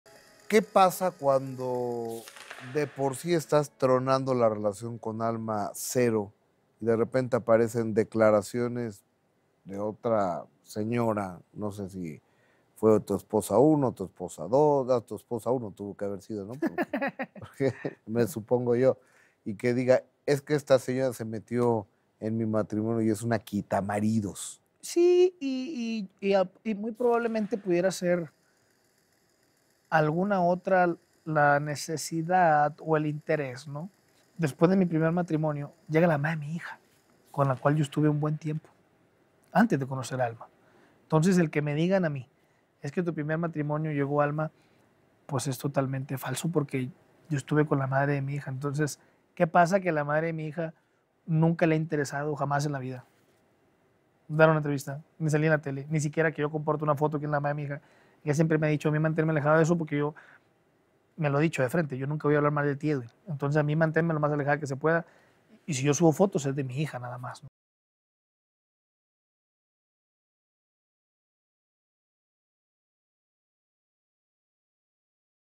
¿Alma Cero es una "quitamaridos"? | EMQCMD
Edwin Luna, el de La Trakalosa, ¿dejó a su familia por Alma Cero?